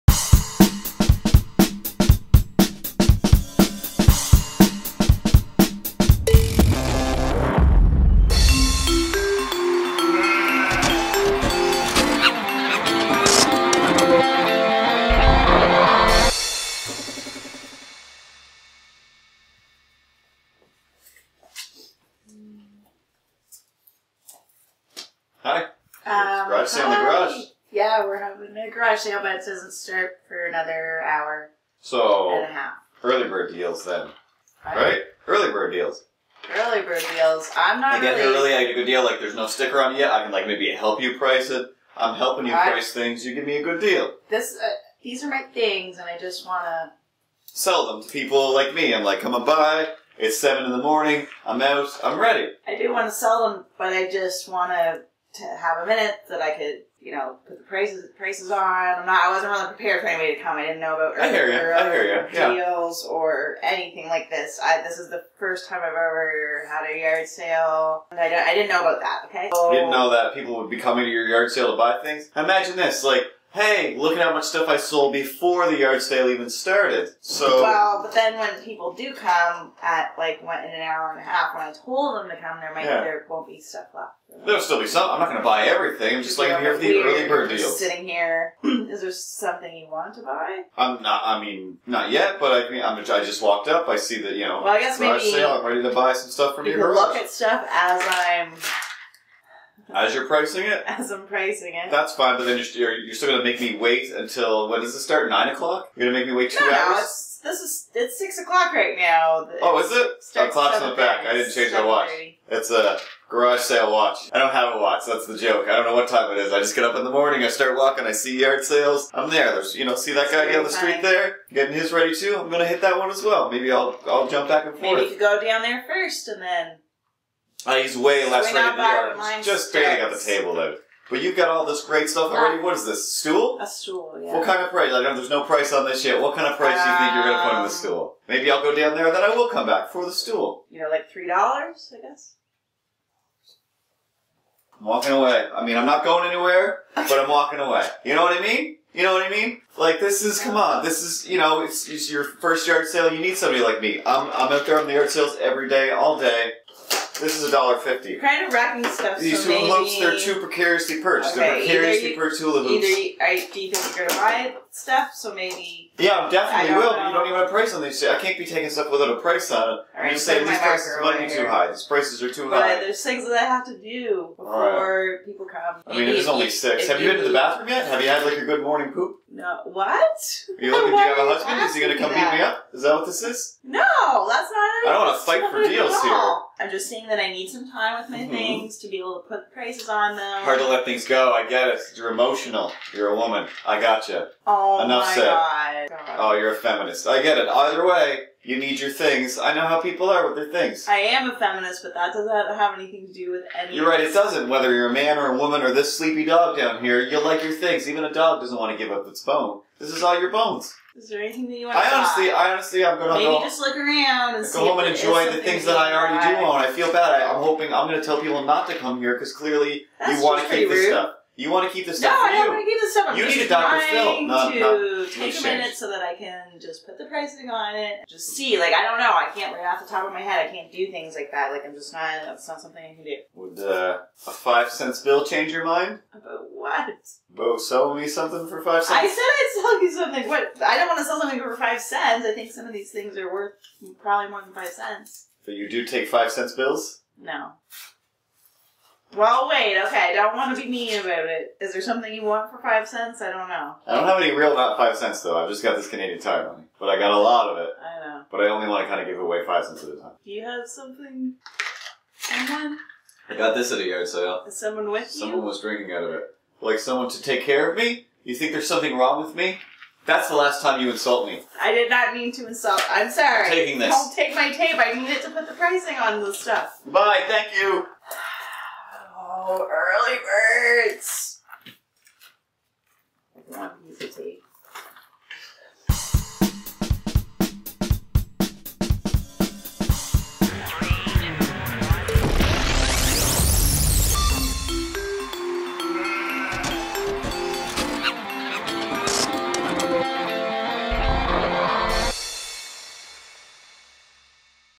Garage Sale Tales Eps 1: Early Birds
From the creators of The Andy & Andy Show, Prepping with Puppets & Blanketfort Stories comes Garage Sale Tales! The real behind the scene stories of the garage sales you know and love, the weirdo's the crazies and all the frustrations improvised true stories of seller's and buyers woe.

IN this episode a buyer comes to the garage sale early! how rude! Watch and see what happens when Early Birds show up in Episode 1: Early Birds the Season Premiere of Garage Sale Tales!